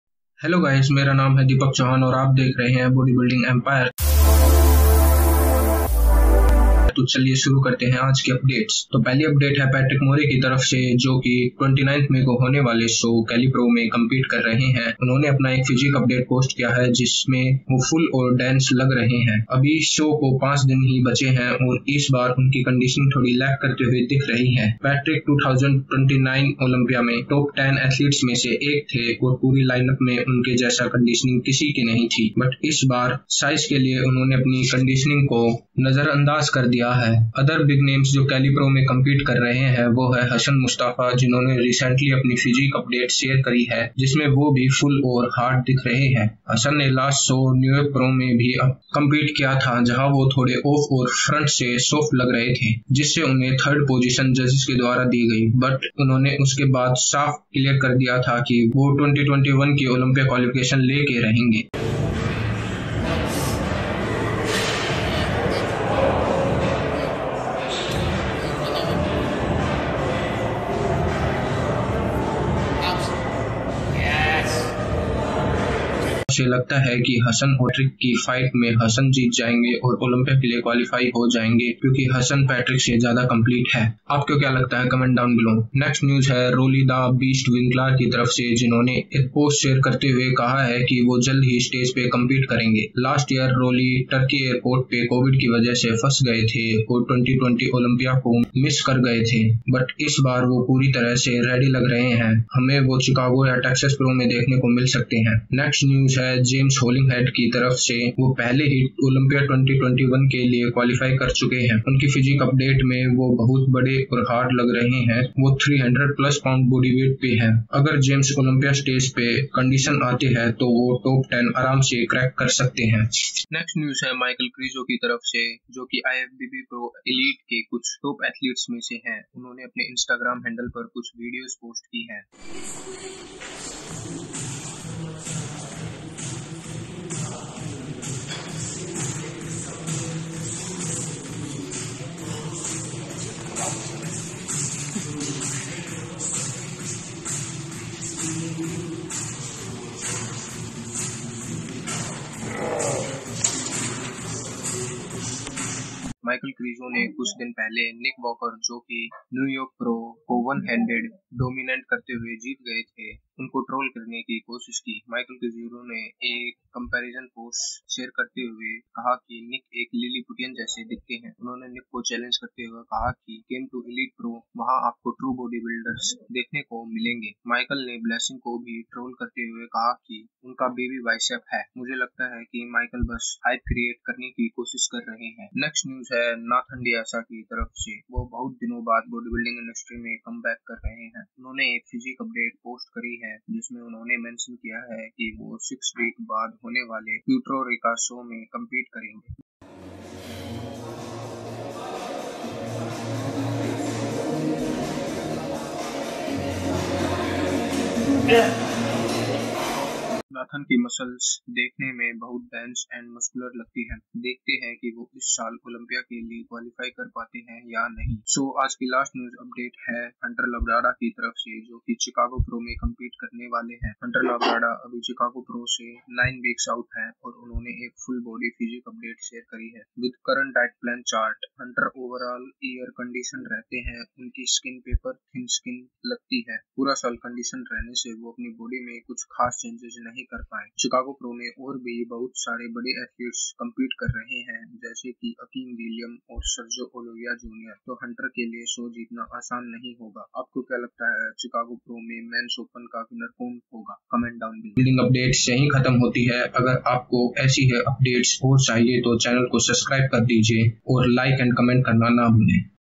HASSAN AND PATRIC IN CALI PRO + ROLLEY COMPETING SOON + KRIZO RIVALRY WITH NICK + NATHAN COMEBACK
Iss  video mein aapko hassan mustafa aur patric moore ki complete physique update ke bare mein btaya gya  h aur vo california pro mein compete bhi karne wale h.
next update rolley winkler ke baare mein h jinhone jald hi pro show mein compete karne ko kha h . next update h james hollingshead ki traf se ek physique upadte jissme vo monster jaise lag rahe h . next update h nathan de asha ki traf se jo bodybuilding industry mmein  jald hi compete karne wale h  .hnext update h michal krizo ki traf se jo nick walker ko troll karte h uaur unko liliputian kahte h.agli update h ek physique update jo ki h hunter labrada ki traf se jo ki chicago pro mein compete kar rahe h .